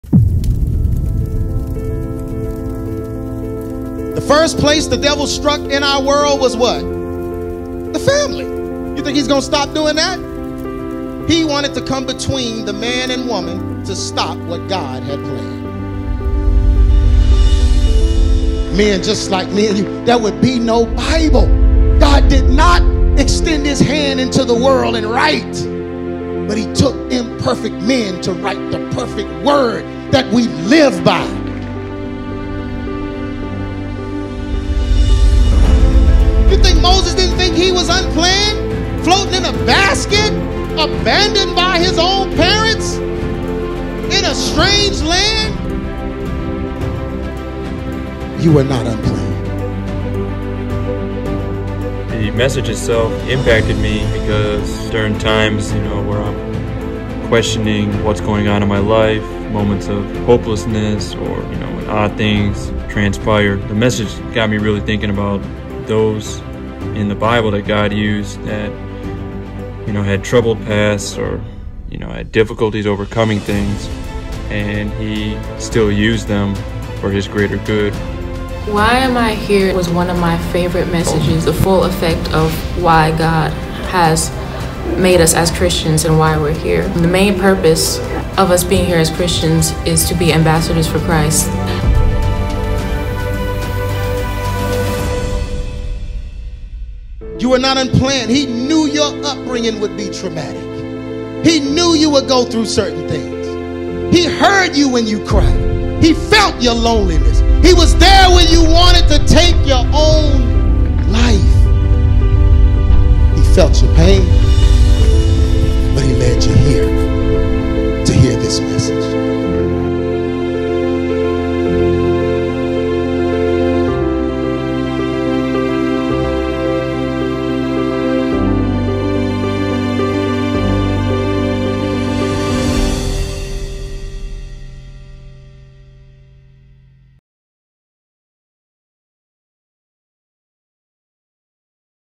EX Ministries Presents: Why Am I Here? with G. Craige Lewis
Why am I here? For centuries, this age-old question has burned in the minds of all mankind. Without purpose, hope is taken away and doubt permeates every area of our being. Separated from God’s purpose for our lives has birthed a culture of godless behavior, perversion and chaos across the earth. If we are to ever have hope, we need to answer the question of why we are here. In this powerful video, G. Craige Lewis provides the answer to this question. This message’s practical answers will provide believers with security and hope. There is a reason why you are here right now! Regardless of where you have come from, no matter your past or present situation, the Lord can show you His true purpose for you and propel you into the future that He has planned for you. It’s time to become who you were created to be.